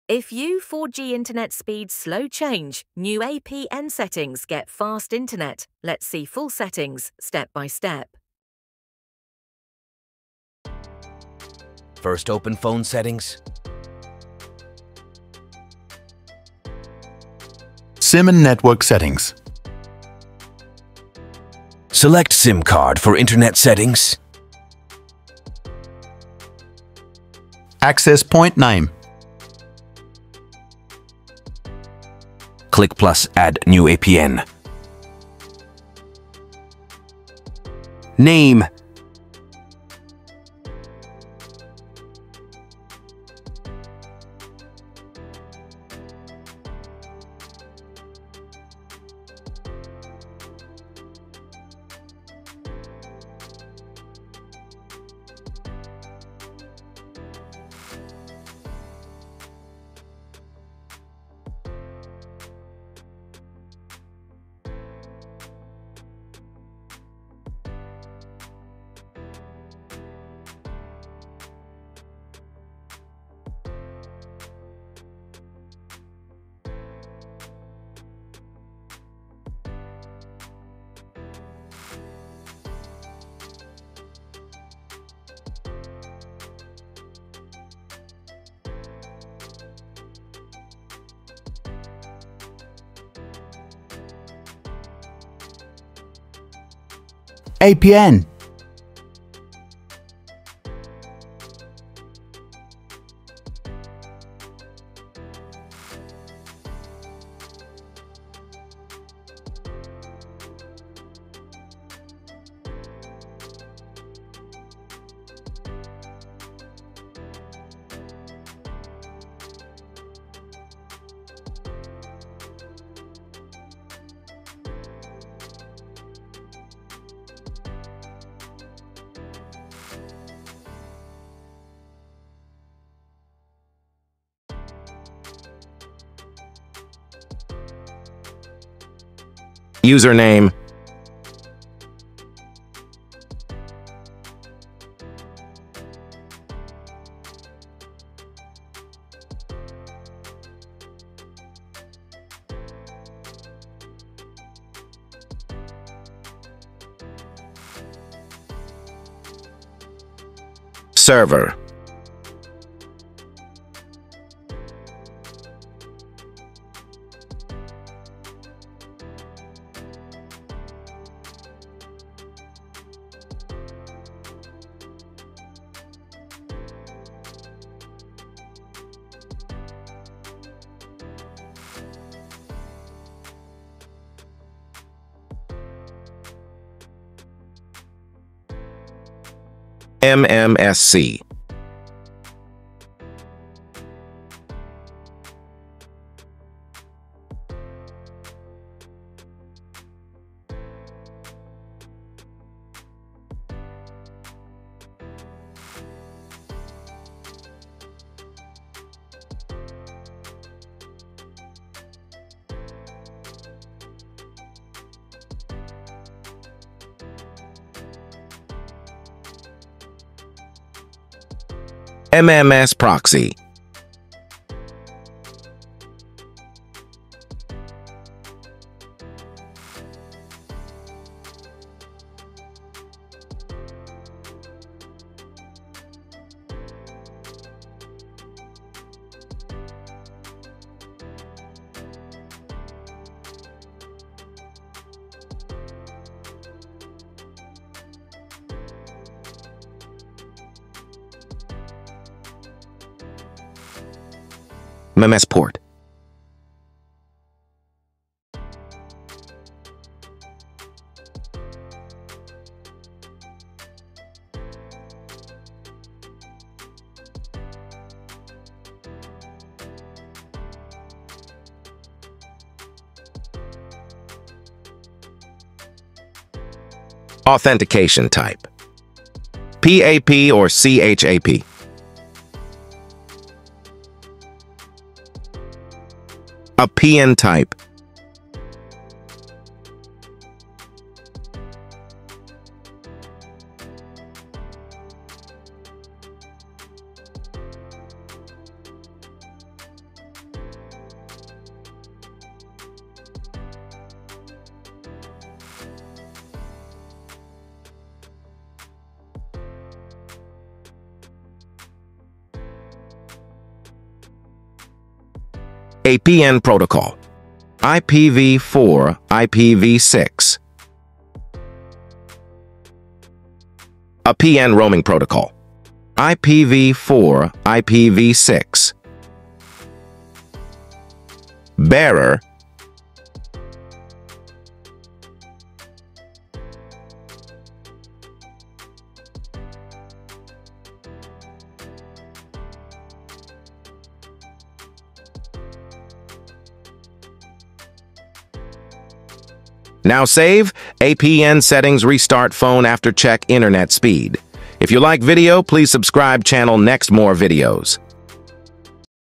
Change Slow network APN Settings for 3G network get 4G LTE Plus speed
If you're experiencing slow data speeds, adjusting your APN settings can sometimes improve connectivity. Here’s how to check and update your APN for faster performance:

For Android:

1. Go to Settings on your device.

2. Select Network & Internet or Connections.

3. Tap Mobile Network  Access Point Names APN

4. Select your current APN or create a new one by tapping Add.